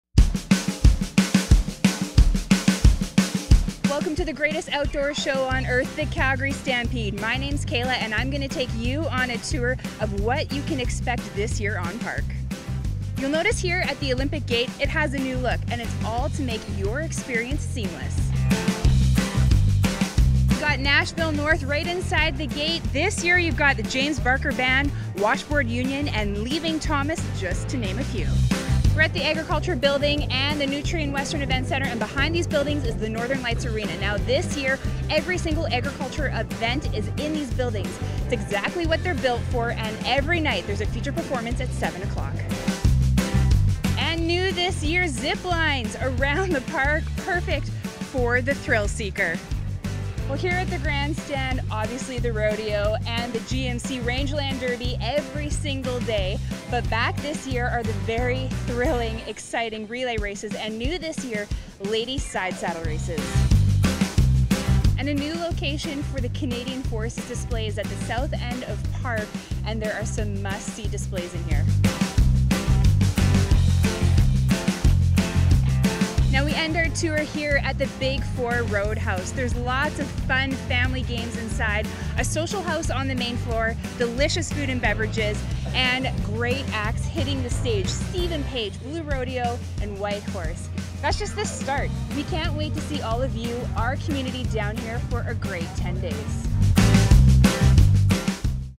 Calgary Stampede Roving Reporter - Walking Tour
Come on a tour with us as we show you some of the new and exciting things on park this year.